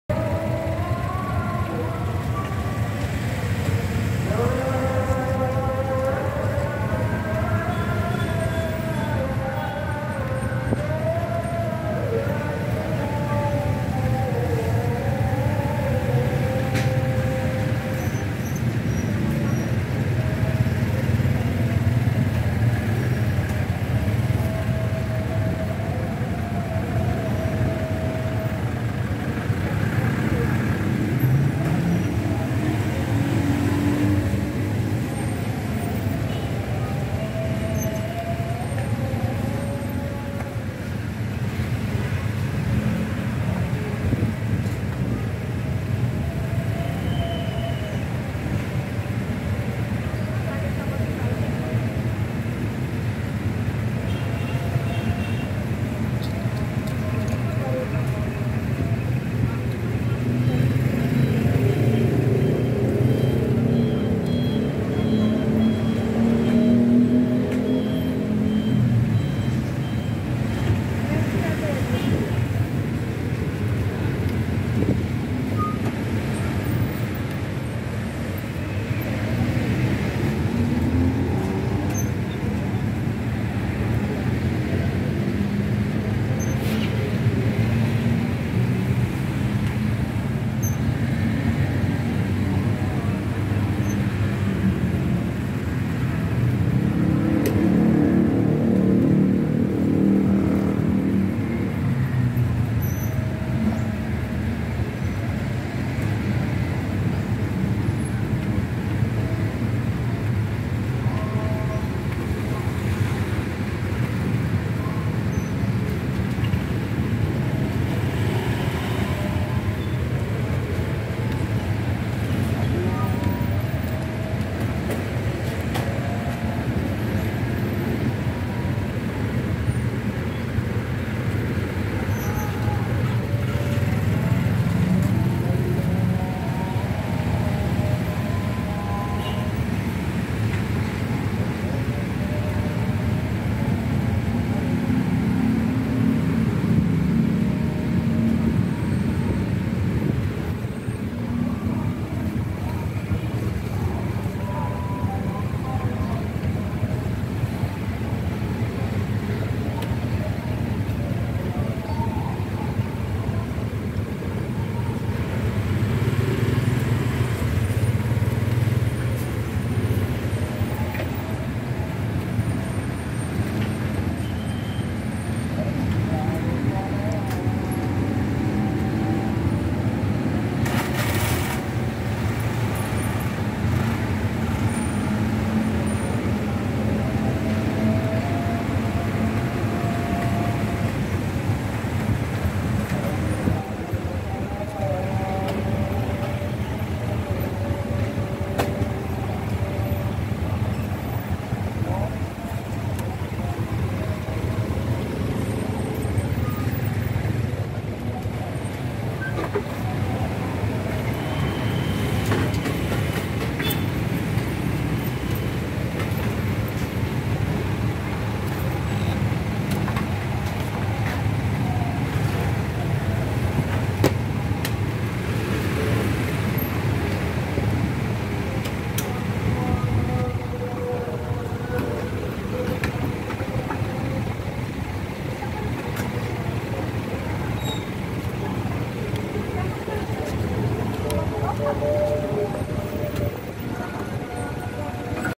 Antri bbm Pertalite di Pom Bensin Poris
Harus sabar mengantri beli bbm pertalite di Pom Bensin Poris Kota Tangerang